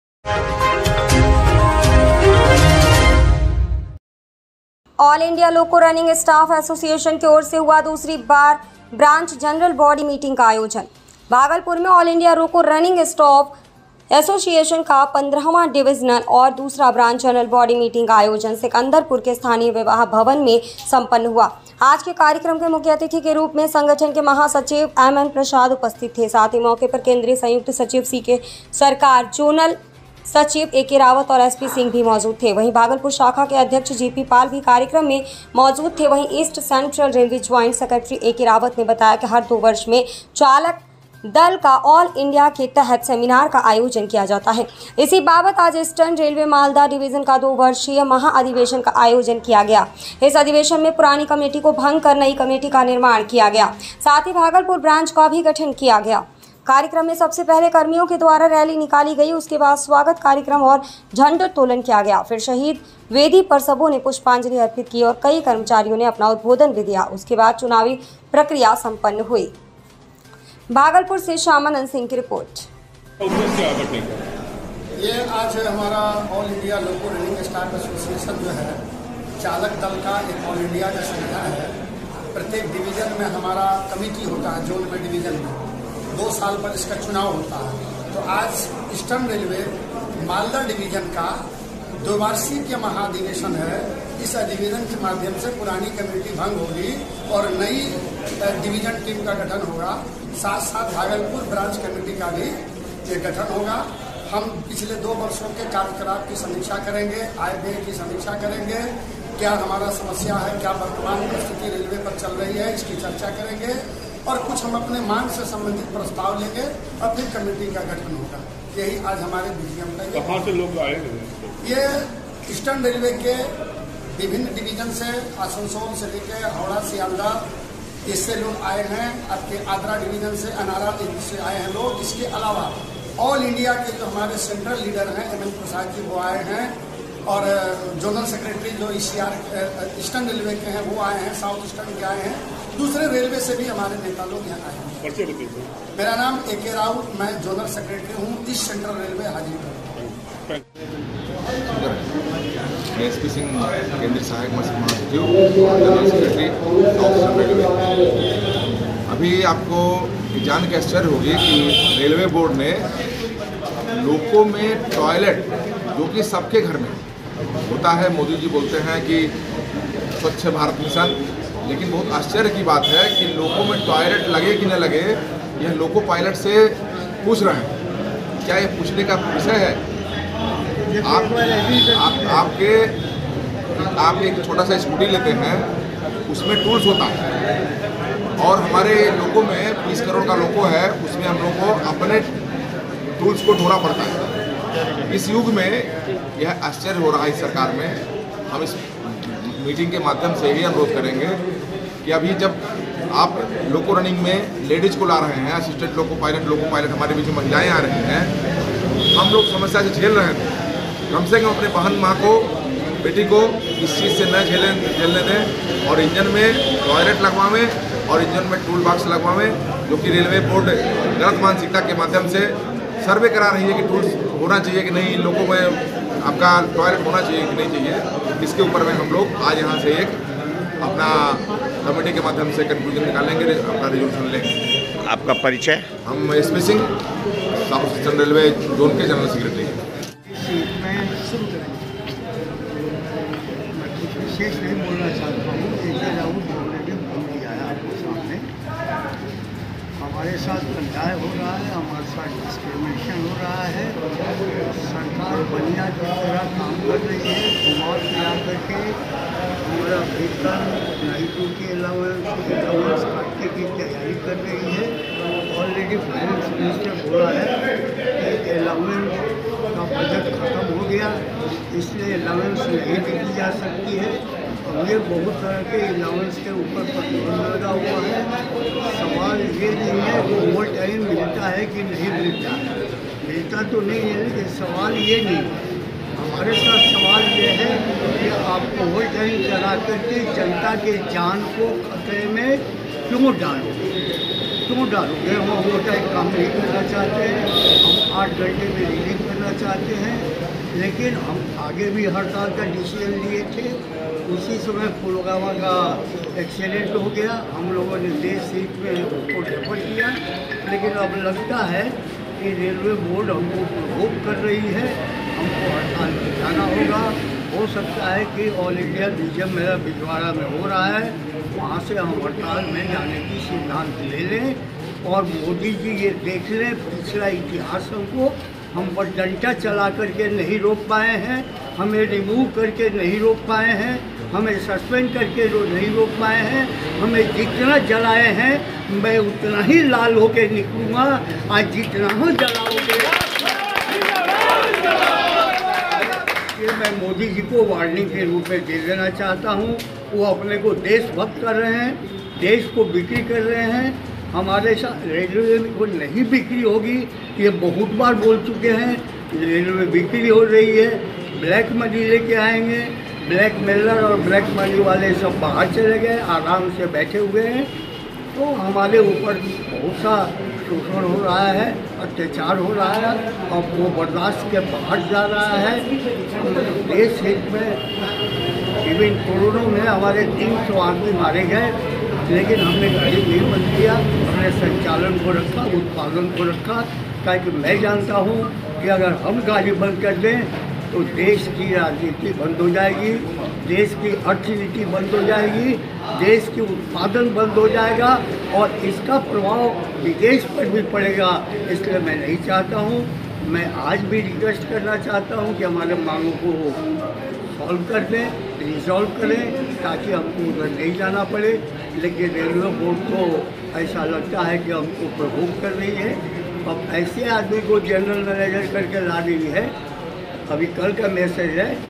ऑल इंडिया लोको रनिंग स्टाफ एसोसिएशन की ओर से हुआ दूसरी ब्रांच जनरल बॉडी मीटिंग का आयोजन
रिपोर्ट-शयामानंद सिह भागलपूर

भागलपुर, ऑल इंडिया लोको रनिंग स्टाफ एसोसिएशन का 15 वां डिविजनल और दूसरा ब्रांच जनरल बॉडी मीटिंग का आयोजन  सिकंदरपुर के स्थानीय विवाह भवन में संपन्न हुआ। आज के कार्यक्रम के मुख्य अतिथि के रुप में संगठन के महासचिव एमएन प्रसाद उपस्थित थे साथ ही मौके पर केंद्रीय संयुक्त सचिव सीके सरकार जोनल सचिव एके रावत और एसपी सिंह भी मौजूद थे वही भागलपुर शाखा के अध्यक्ष जेपी पाल भी कार्यक्रम में मौजूद थे वही ईस्ट सेंट्रल रेलवे ज्वाइंट सेक्रेट्री एके रावत ने बताया कि हर 2 वर्ष में  चालक दल का ऑल इंडिया के तहत सेमिनार का आयोजन किया जाता है इसी बाबत आज ईस्टर्न रेलवे मालदा डिविजन का 2 वर्षीय महा अधिवेशन का आयोजन किया गया इस अधिवेशन में पुरानी कमेटी को भंग कर नई कमेटी का निर्माण किया गया साथ ही भागलपुर ब्रांच का भी गठन किया गया साथ ही साथ कई बिंदुओं पर  अपनी मांगों को लेकर एवं कार्य को सुगमता से चलाने को लेकर समीक्षात्मक वार्ता भी की गई ।आज के इस अधिवेशन में  स्टर्न रेलवे के विभिन्न डिवीजन के कर्मी भी मौजूद थे।

कार्यक्रम में सबसे पहले कर्मियों के द्वारा रैली निकाली गई उसके बाद स्वागत कार्यक्रम और झंडोत्तोलन किया गया फिर शहीद वेदी पर सबों ने पुष्पांजलि अर्पित किया और कई कर्मचारियों  ने अपना उद्बोधन भी दिया उसके बाद चुनावी प्रक्रिया संपन्न हुई।

बाईट:- जोनल सेक्रेट्री ईस्ट सेंट्रल रेलवे एके रावत